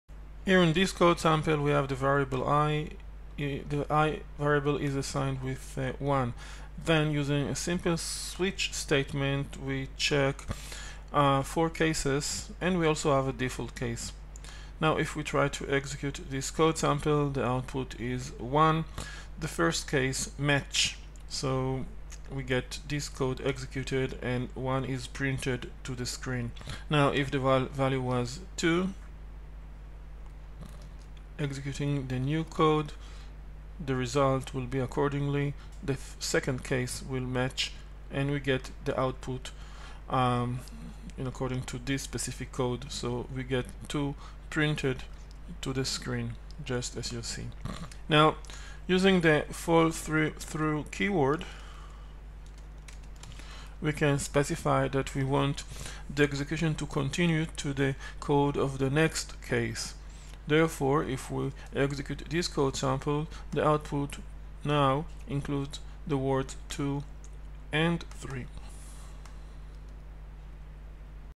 The Swift fallthrough Keyword in Switch Statement
Using the break keyword we can stop the execution of a specific loop or a switch statement. Using the continue keyword in a loop we can continue to the next iteration.